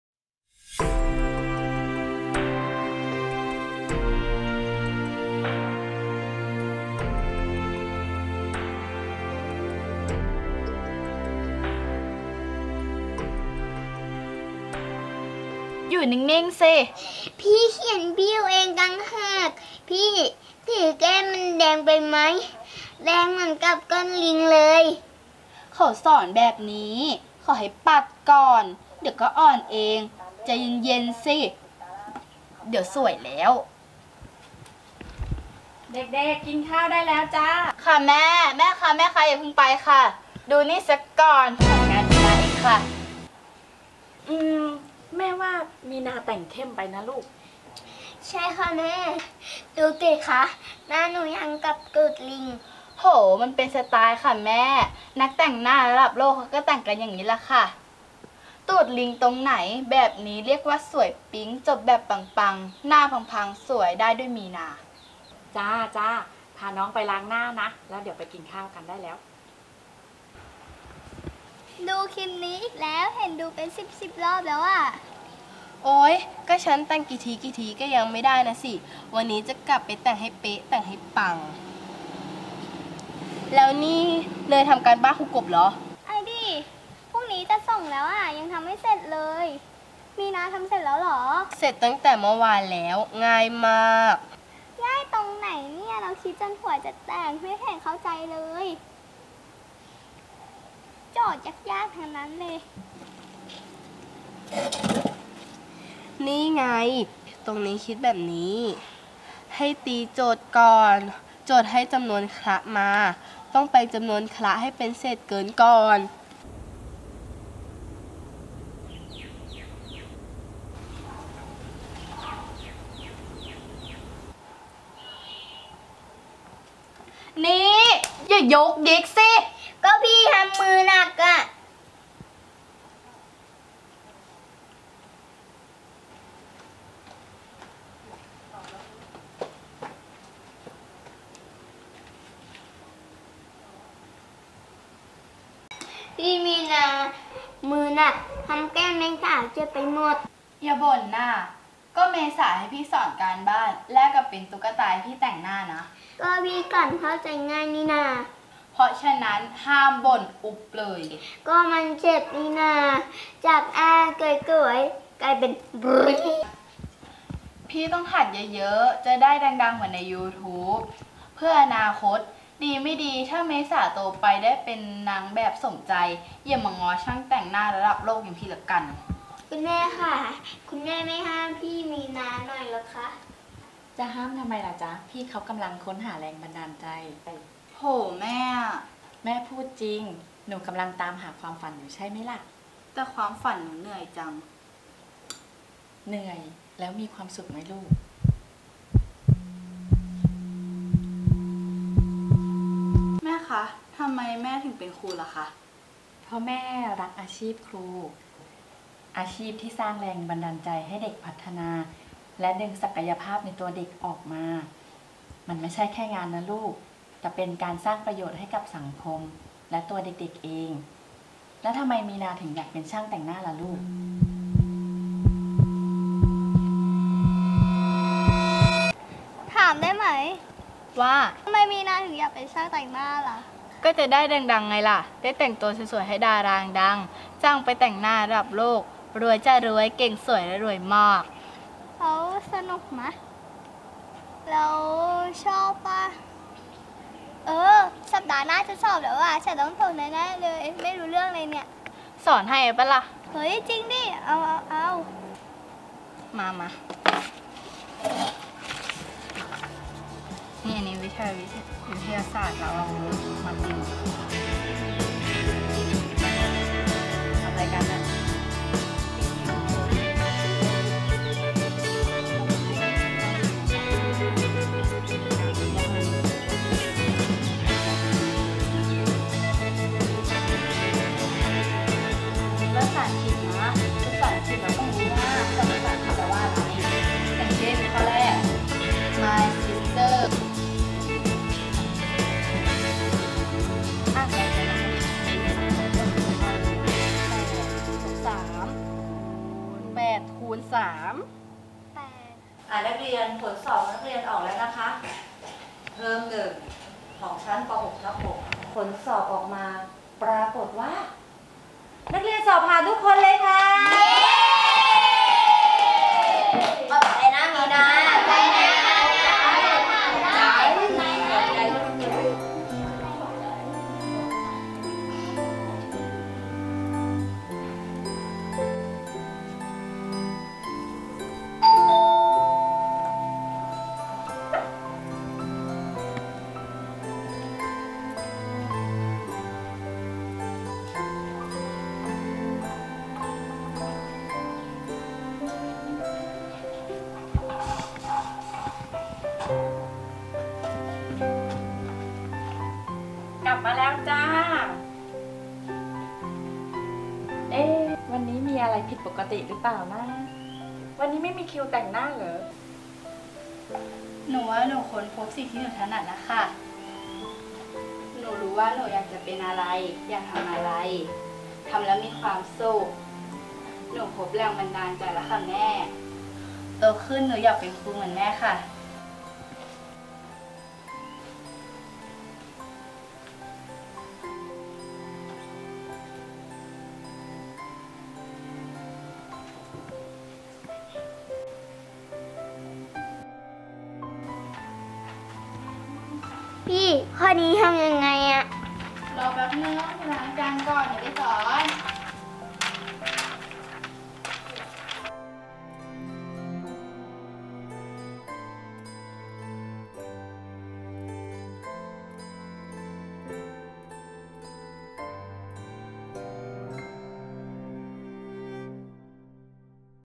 ภาพยนตร์สั้นโรงเรียนวัดศรีสุวรรณคงคาราม เรื่อง ค้นพบสิ่งที่ชอบ เติมเต็มสิ่งที่ใช่
ภาพยนตร์สั้นโรงเรียนวัดศรีสุวรรณคงคาราม(ราษฎร์สงเคราะห์) ภายใต้แนวคิด รู้จักตน ค้นพบตนเอง เรื่อง ค้นพบสิ่งที่ชอบ เติมเต็มสิ่งที่ใช่